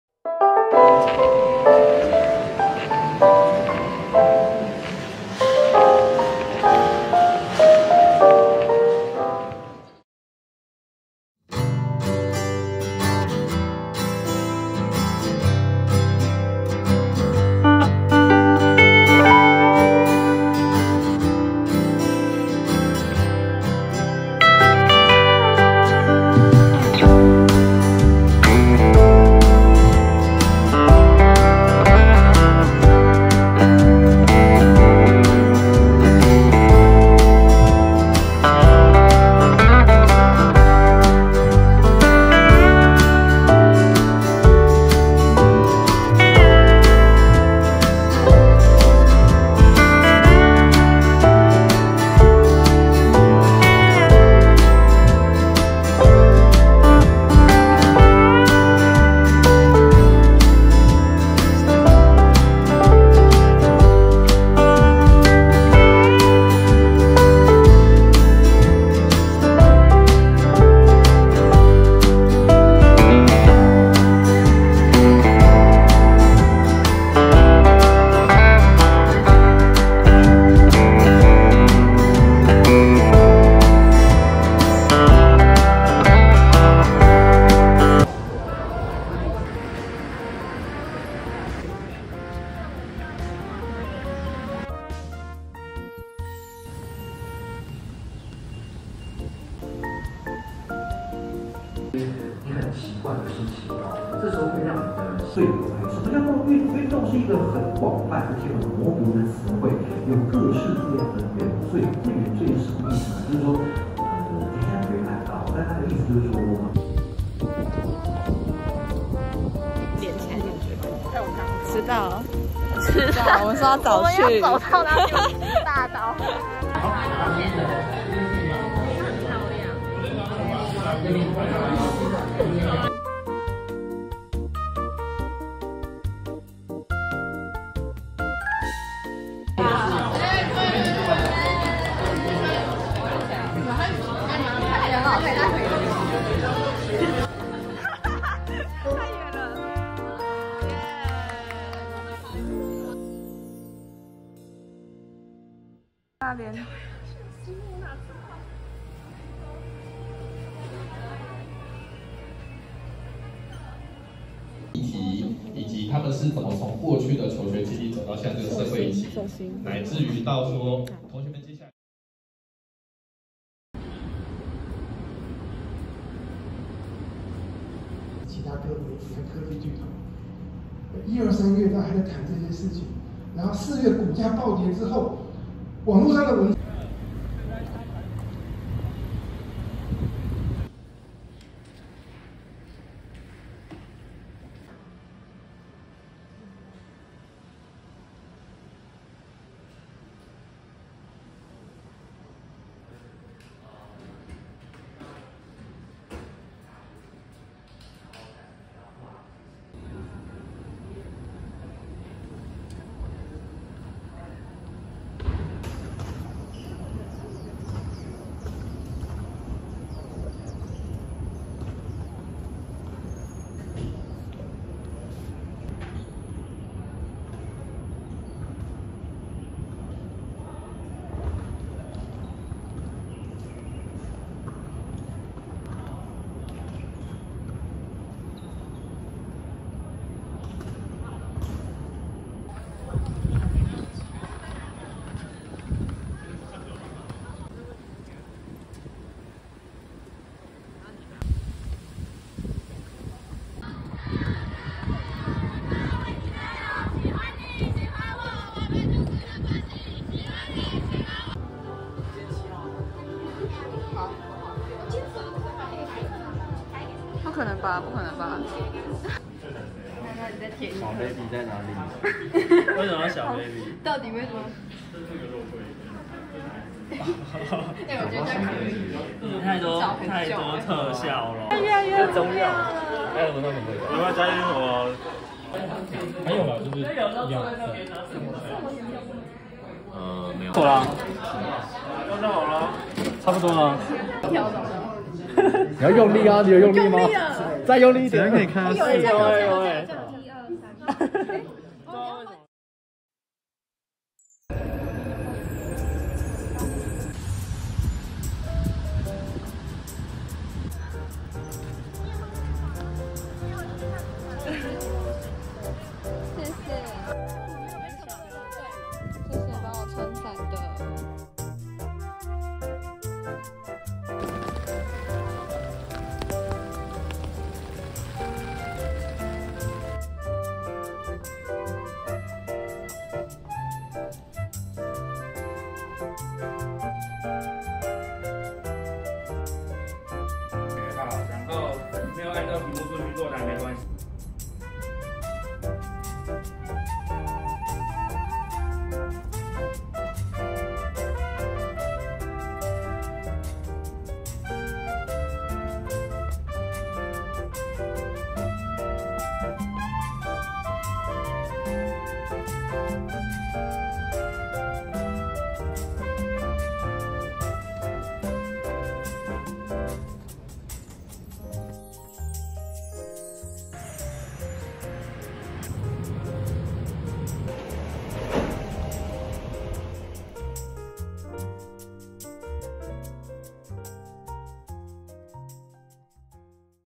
NTU VLOG｜台大生的一週日常：學校適合唸書的角落、學習規劃會談、臨時抱佛腳的小考
Film: iPhone13 mini
Edit: VLLO